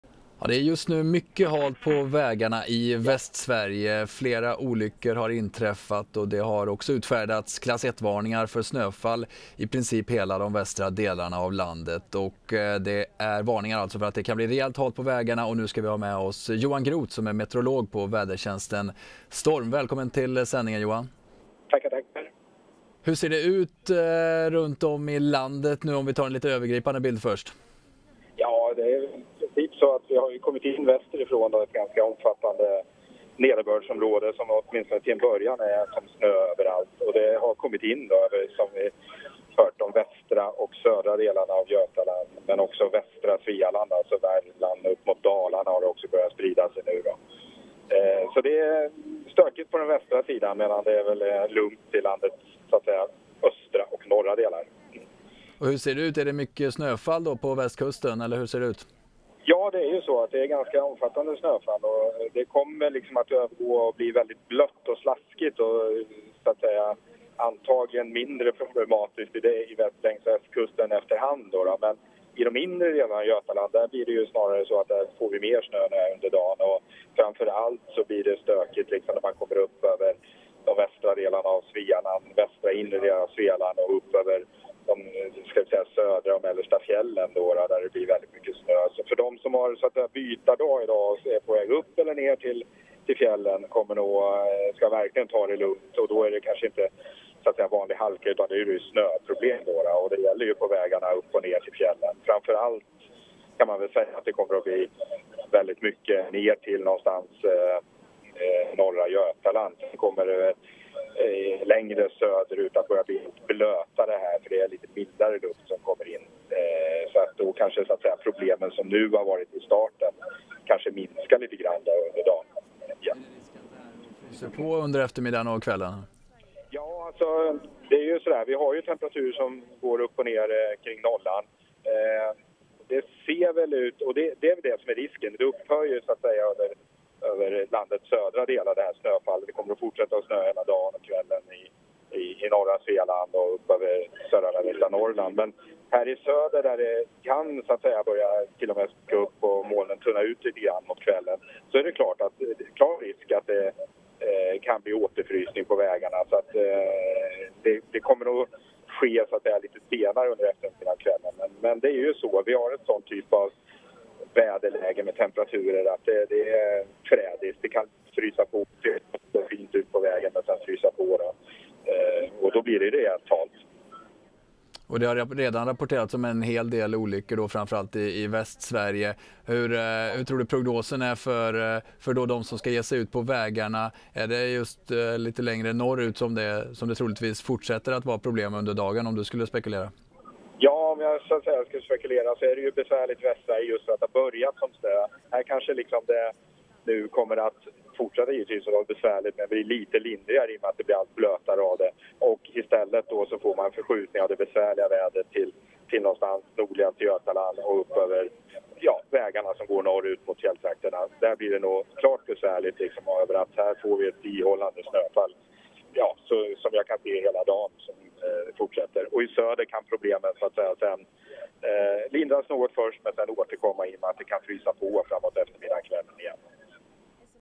Flera olyckor efter snöoväder på västkusten
Johan Groth på Storm: "Ett omfattande snöfall"